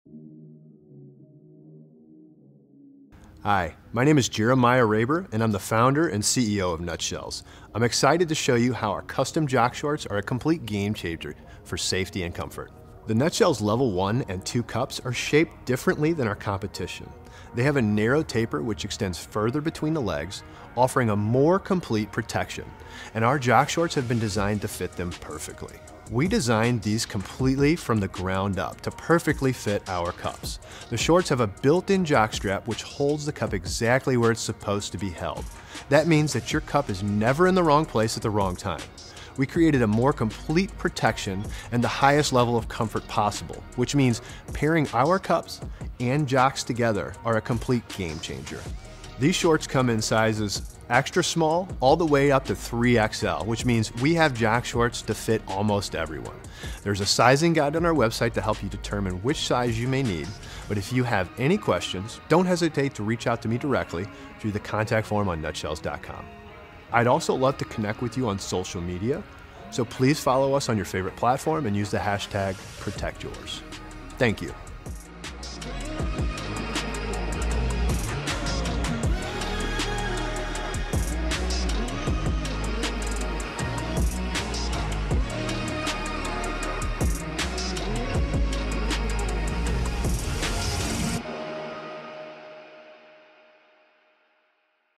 All Sport Jock Shorts | NUTSHELLZ
Hi, My name is Jeremiah Raber, and I am the Founder and CEO of Nutshellz. I’m excited to show you how our custom jock shorts are a complete game changer for safety and comfort.

The Nutshellz level 1 and 2 cups are shaped differently than our competition…they have a narrow taper which extends further between the legs, offering a more complete protection and our Jock shorts have been designed to fit them perfectly.

We designed these completely from the ground up to perfectly fit our cups.  The shorts have a built-in jock strap which holds the cup exactly where it’s supposed to be held.…that means that your cup is never in the wrong place at the wrong time. We created a more complete protection and the highest level of comfort possible which means, pairing our cups and jocks together are a complete game changer.

These shorts come in sizes X-Small all the way up to 3 XL which means we have jock shorts to fit almost everyone!  There is a sizing guide on our website to help you determine which size you may need, but if you have any questions don’t hesitate to reach out to me directly through the contact form on Nutshellz.com

I’d also love to connect with you on social media so please follow us on your favorite platform and use the hashtag protectyours. Thank you!